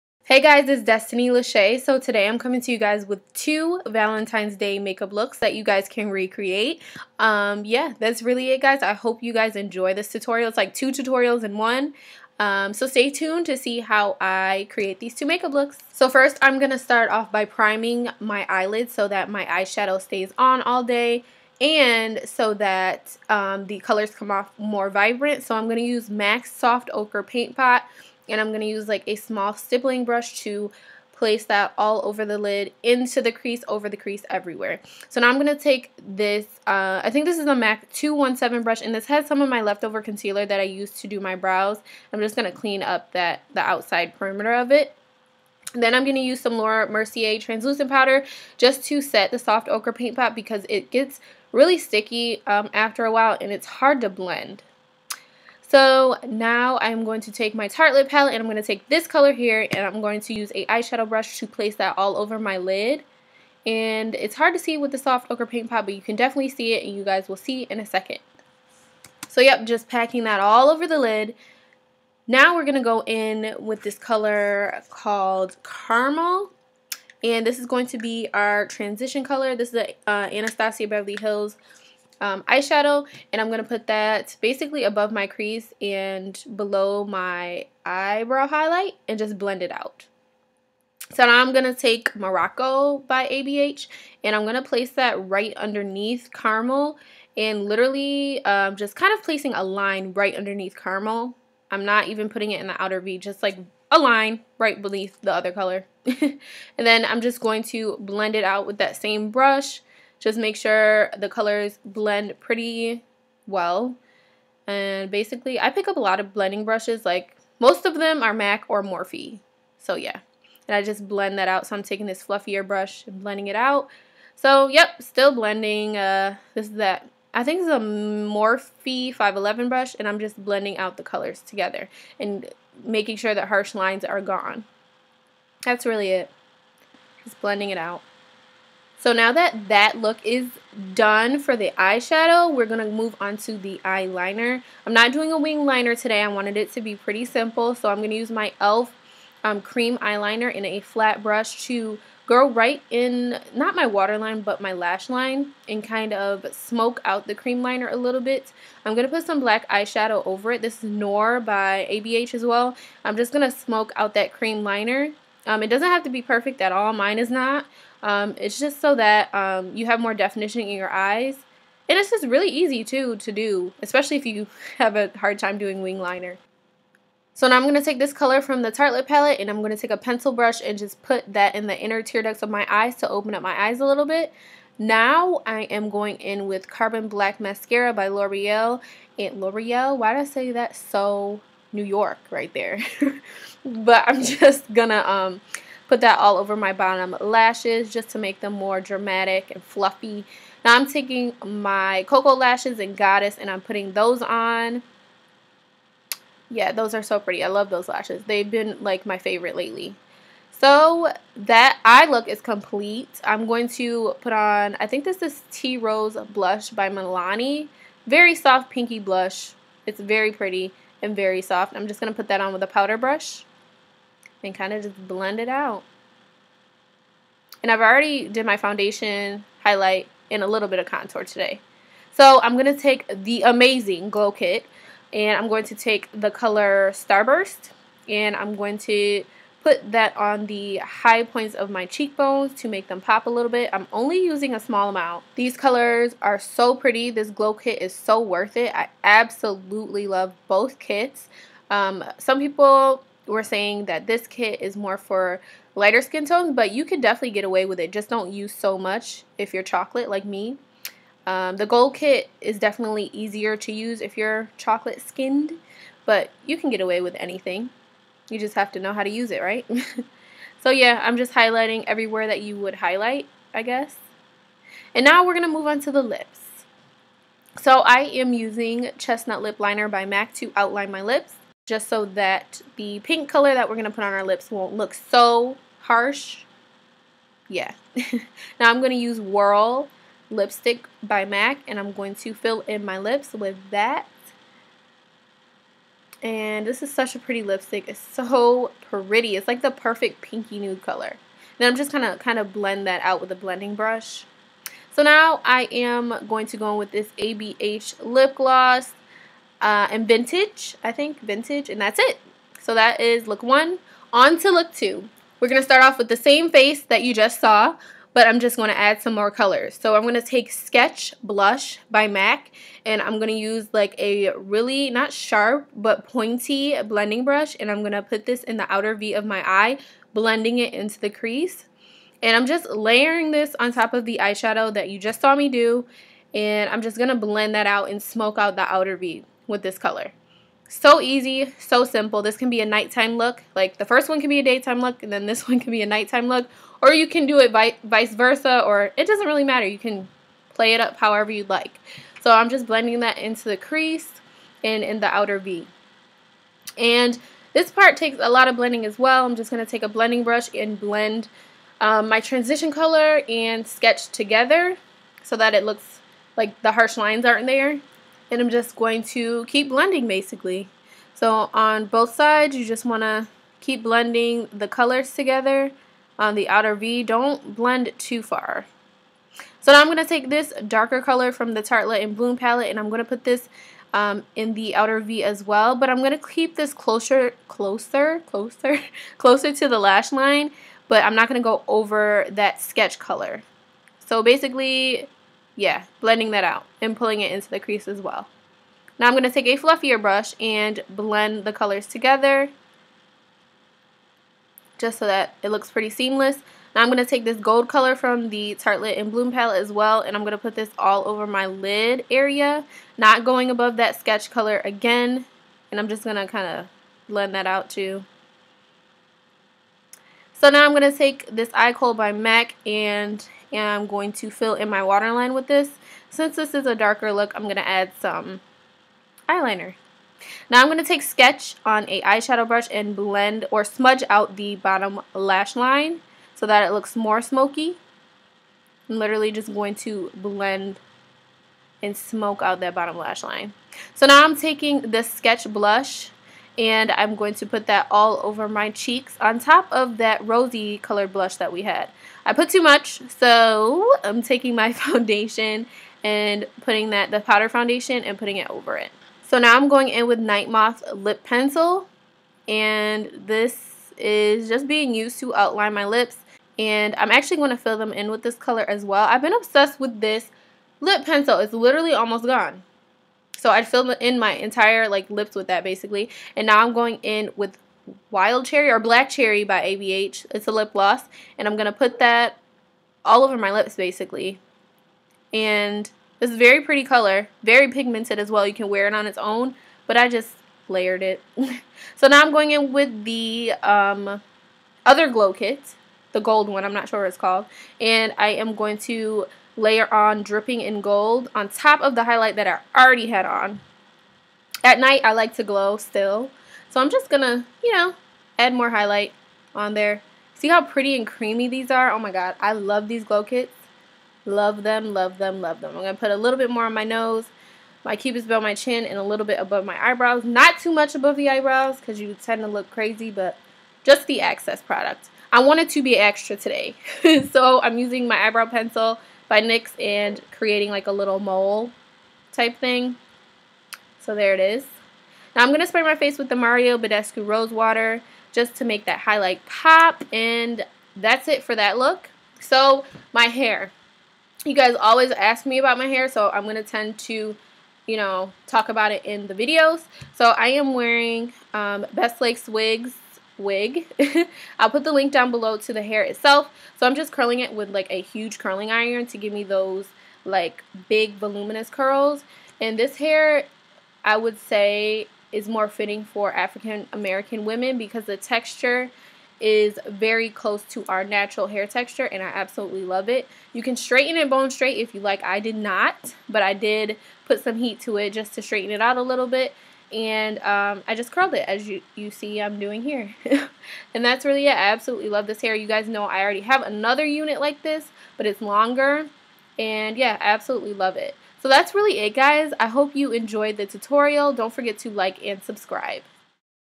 Day to Night Makeup Looks Ft. Kinky Straight Lace Wigs
Style NO： Gsw147
Hair Texture: kinky Straight Hair
Hair Color: #1 jet black
Hair Length: 16inches
Cap: Cap11 glueless full lace wig